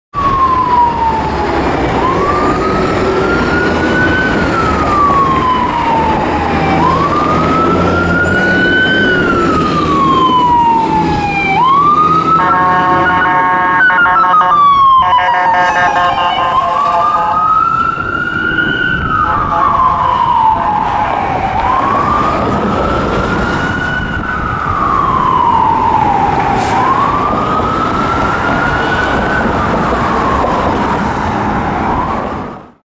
PFD Medic 53B Responding
Philadelphia Fire Department Medic 53B responding to a medical assignment on John F Kennedy Blvd.

Medic 53B - 2012 International TerraStar/Horton (EX-Medic 11) (Now Medic 155E)

Recorded: 3/2018